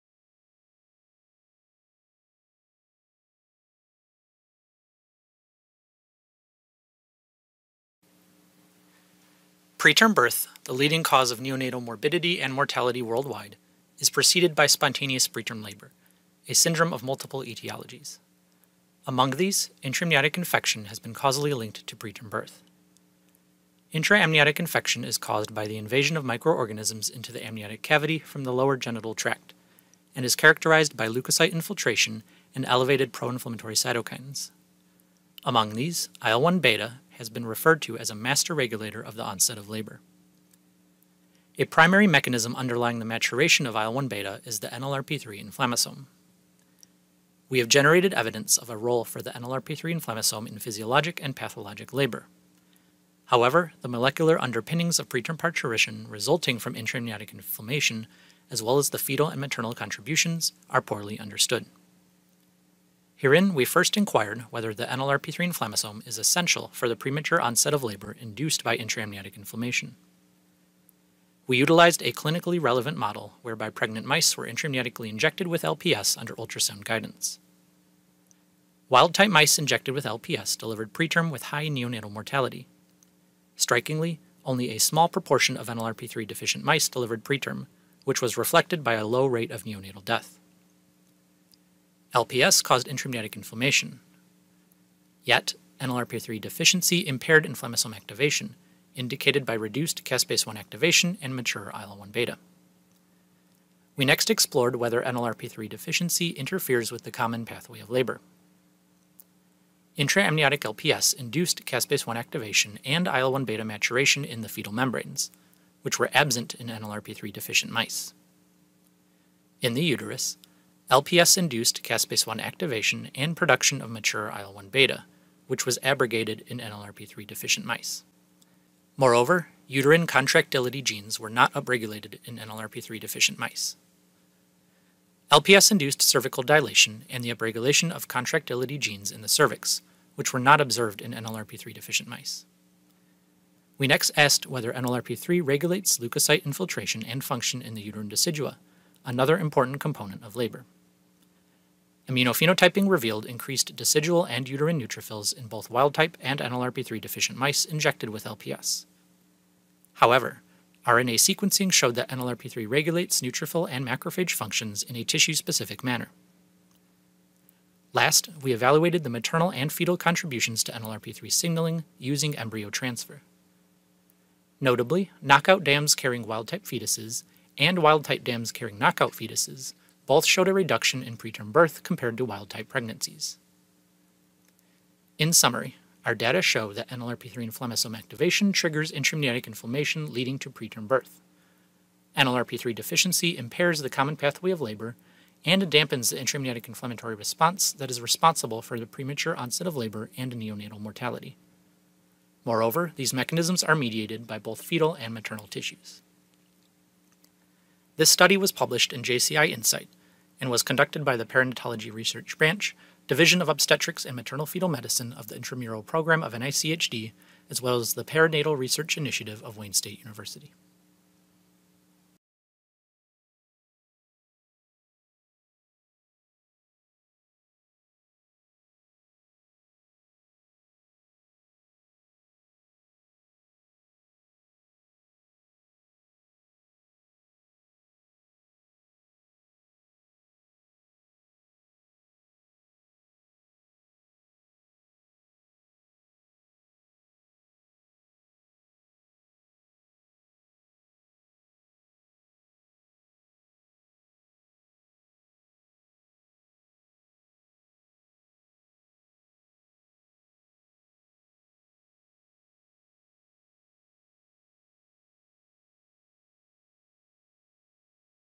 NLRP3 signaling and preterm labor and birth
In this episode, the authors explain that fetal and maternal NLRP3 signaling participates in the intra-amniotic inflammatory response and common pathway of labor leading to premature parturition through inflammasome-associated and -independent mechanisms.